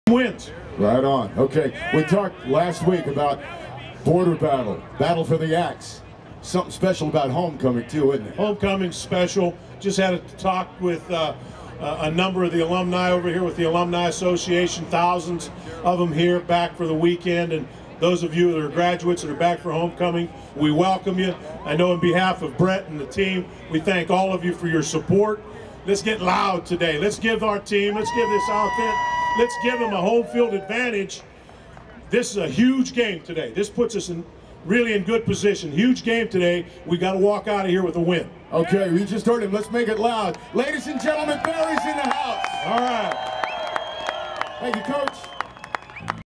UWGameday: Barry Alvarez
Barry Alvarez speaks at Badgerville